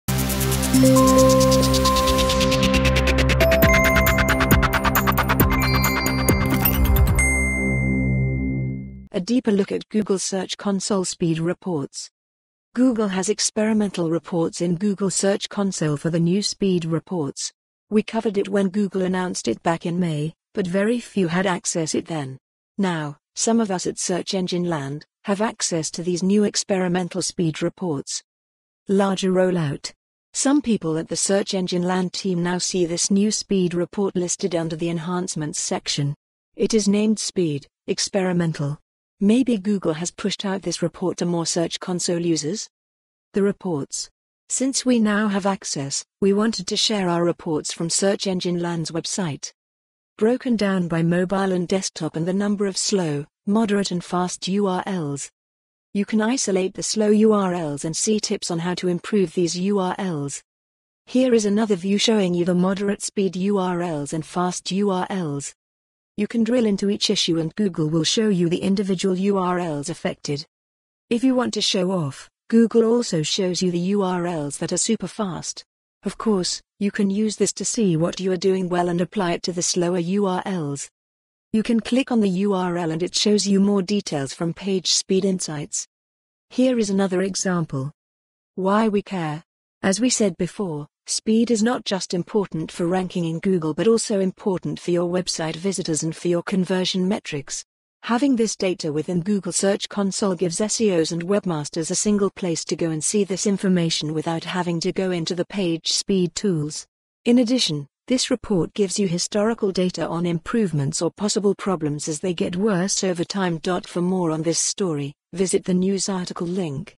A deeper look at Google Search Console Speed Reports
Google has experimental reports in Google Search Console for the new speed reports. We covered it when Google announced it back in May, but very few had access it then. Now, some of us at Search Engine Land, have access to these new experimental speed reports.

Larger roll out. Some people at the Search Engine Land team now see this new speed report listed under the “Enhancements” section. It is named “Speed (experimental).” Maybe Google has pushed out this report to more Search Console users?

The reports. Since we now have access, we wanted to share our reports from Search Engine Land’s web site.

Broken down by mobile and desktop and the number of slow, moderate and fast URLs.

You can isolate the slow URLs and see tips on how to improve these URLs.

Here is another view showing you the moderate speed URLs and fast URLs.

You can drill into each issue and Google will show you the individual URLs affected.

If you want to show off, Google also shows you the URLs that are super fast. Of course, you can use this to see what you are doing well and apply it to the slower URLs.

You can click on the URL and it shows you more details from PageSpeed Insights.

Here is another example.

Why we care. As we said before, speed is not just important for ranking in Google but also important for your website visitors and for your conversion metrics. Having this data within Google Search Console gives SEOs and webmasters a single place to go and see this information without having to go into the PageSpeed tools. In addition, this report gives you historical data on improvements or possible problems as they get worse over time.